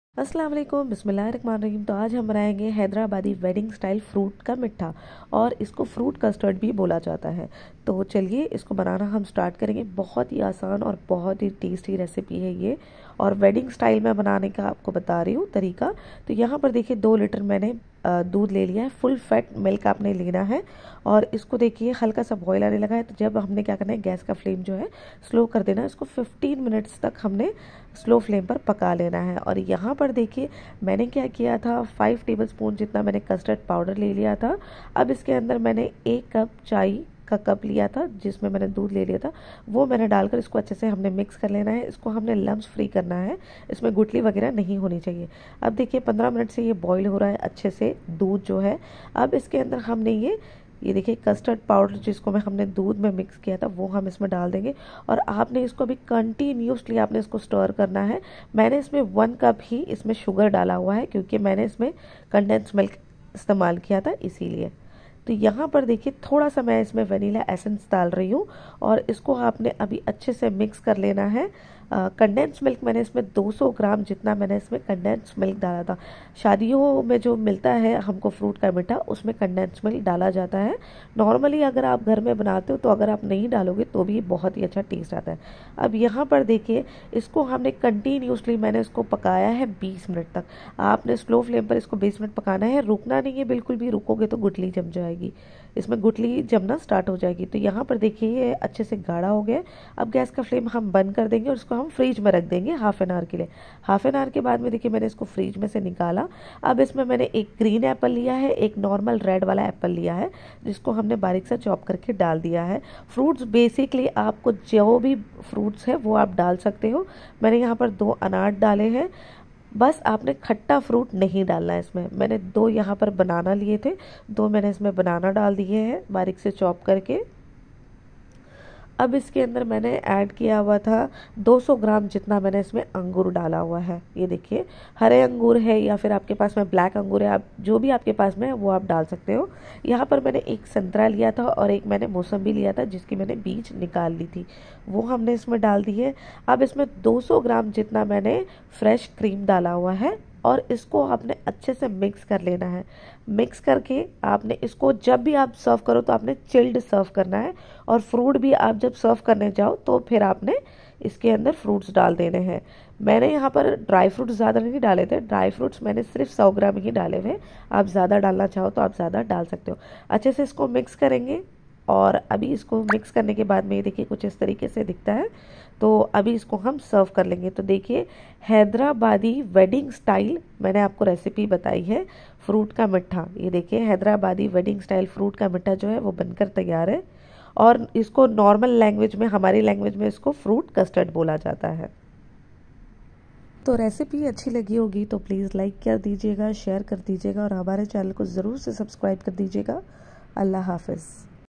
Hyderabadi Shadiyo Wala Fruit Ka Meetha | fruit custard HAFSA KITCHEN | HINDI / URDU
fruit ka meetha
ingredients
2 liter milk
250 gram sugar
100 gram milkmaid
259 gram cream
4 tsp custard powder
1 tsp vanilla essence
50 gram kishmish
50 gram Kaju
50 gram Badam
6 medium size Banana
8 sapota
1 mosambi
1 pomegranate (Anar)
2 apple
2 kiwi
250 gram grapes